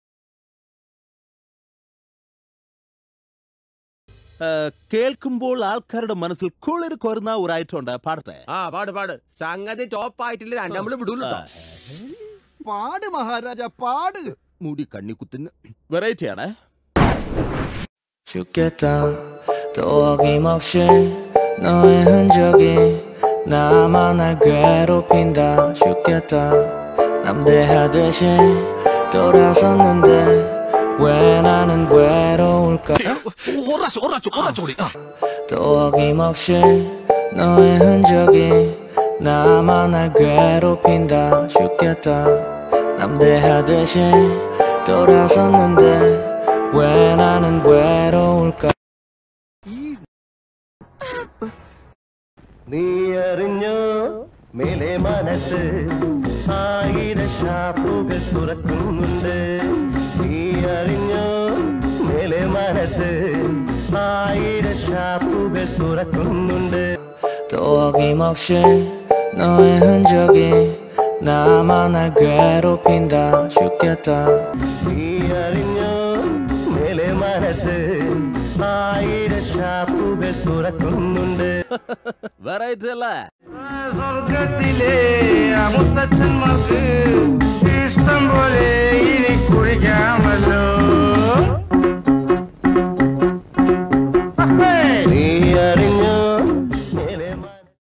കൊറിയനിലേക്ക് മാറ്റിയാൽ അറിയില്ലെന്ന് കരുതിയോടാ കൊറിയക്കാരാ😃😛 | iKON Song Tune Copied| Troll Video
|  iKON Song Tune Copied from Malayalam? | iKON ട്യൂൺ മലയാളത്തിൽ നിന്ന് കോപ്പി ചെയ്തതാണോ? | Troll Video